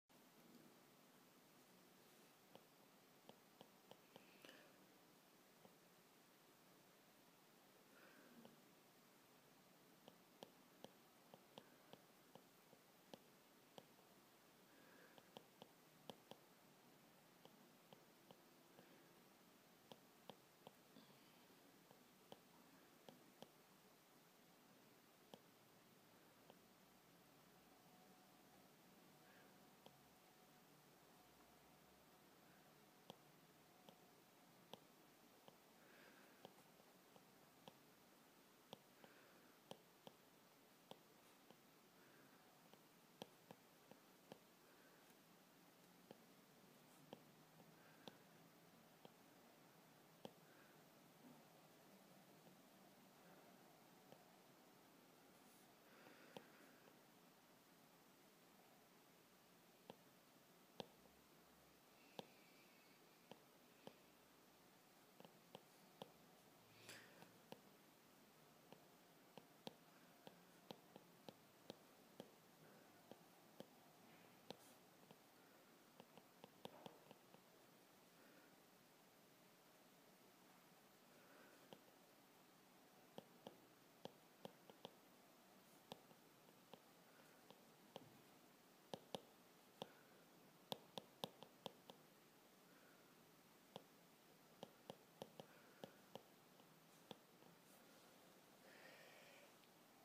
iPad Pro Pencil Sketching mit Procreate App
This close-up of the pencil shows in realtime the sketching of an eye.